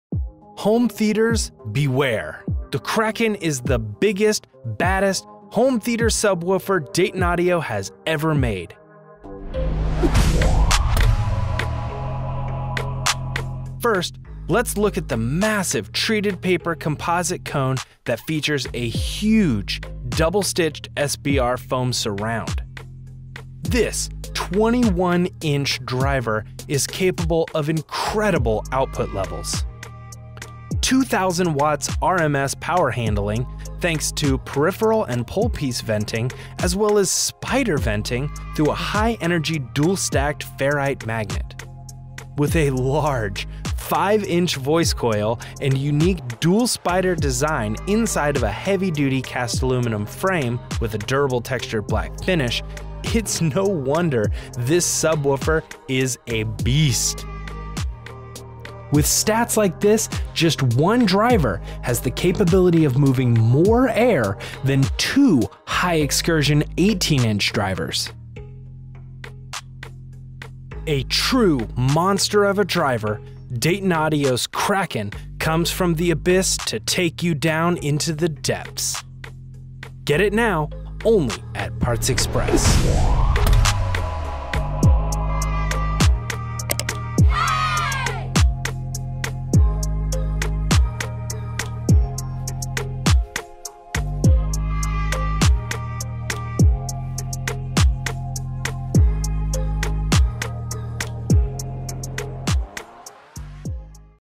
[THE KRAKEN] Huge 21" Home Theater Subwoofer from Dayton Audio
Dayton Audio's HTS545HE-4 21" Kraken high output home theater subwoofer is built for the deepest bass, maximum SPL, and low distortion. Appropriately named the Kraken, this beast of a driver is capable of moving extreme amounts of air with outstanding control and power. With a potent ferrite motor, 5" voice coil, rigid treated paper cone, dual spiders, rugged cast aluminum frame, 95.6 dB sensitivity, and 42 mm of peak-to-peak excursion this driver will seriously shake your home theater to its foundation. The Kraken is the master of the deep!